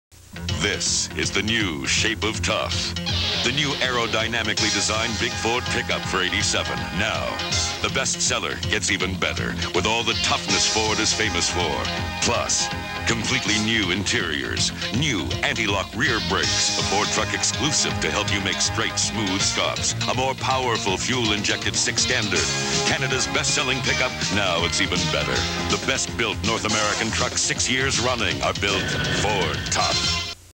Ford Truck Commercial - 1986
Tape 0036 - Sledge Hammer - Beta III - 4.5 Hrs

Cue Card Listings:

Sledge Hammer
0164 “Hammer Gets Nailed”
1606 “They Shoot Hammers Don’t they”
2731 “Magnum Farce” poor
4436 “ The OLD man and the Sledge”
5232 “To Live and Die on TV”
6025

Tape 0036 features 5 episodes of Sledge Hammer taped from Global TV in Toronto sometime in 1986. All commercials are intact. The episode denoted as Poor has a serious warble too it, and a very interesting look. Must have originated from original record.